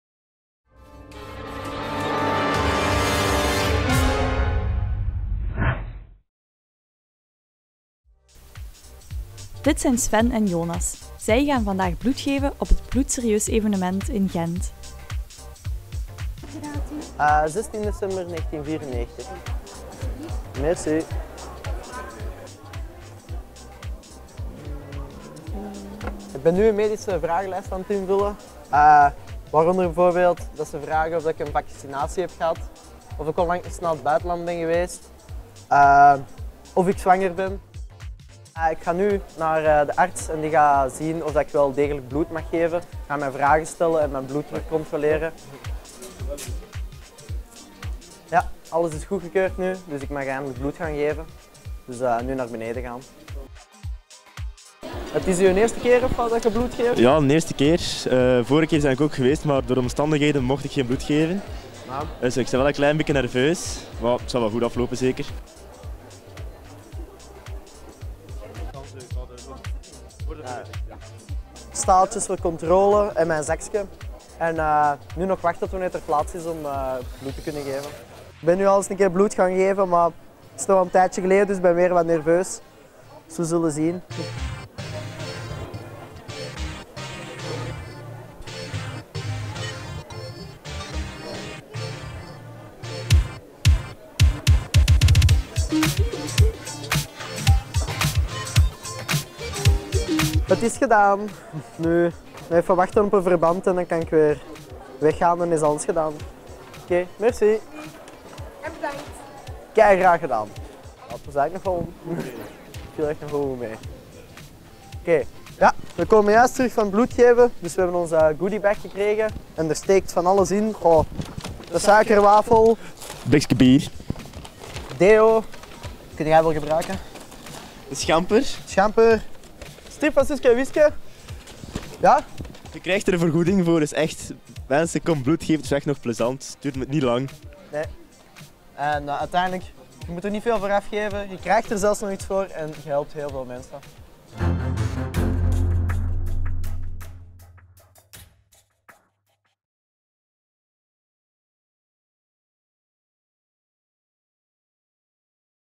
Bloedserieuze jongeren
Maar liefst 500 jongeren kwamen woensdag 12 maart bloed geven op het Bloedserieus evenement in Gent. Maar wil jij zelf ook eens je steentje bijdragen? Kijk dan hier hoe het in zijn werk gaat. Wij volgden Sven en Jonas tijdens hun heldendaad.

Muziek bewerkt en beschikbaar onder een Creative Commons Licentie